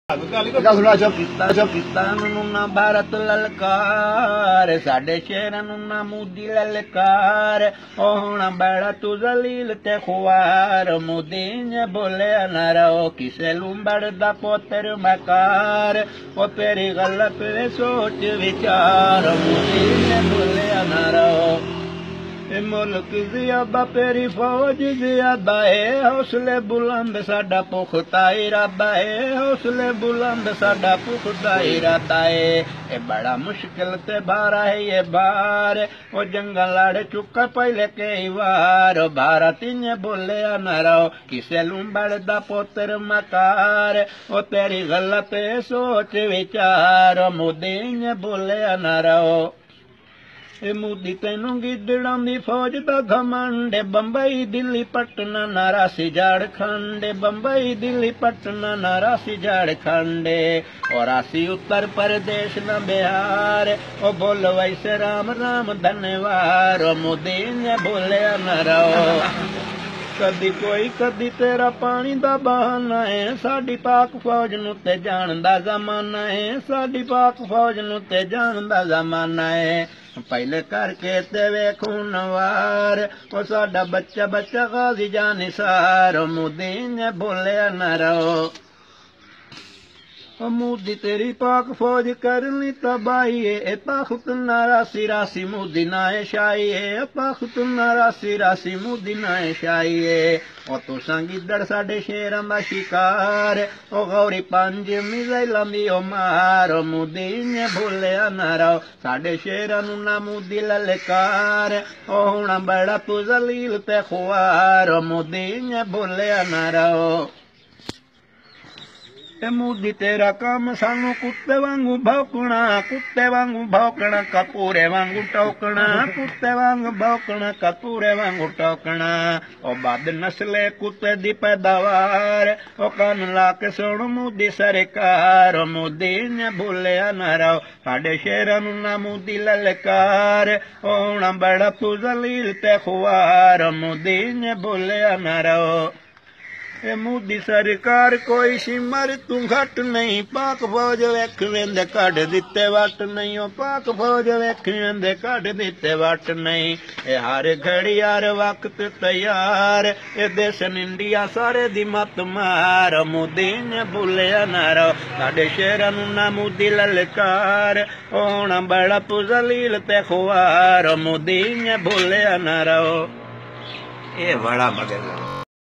Modi Ghazal in Punjabi language
A Punjabi man singing a GHazal for Modi.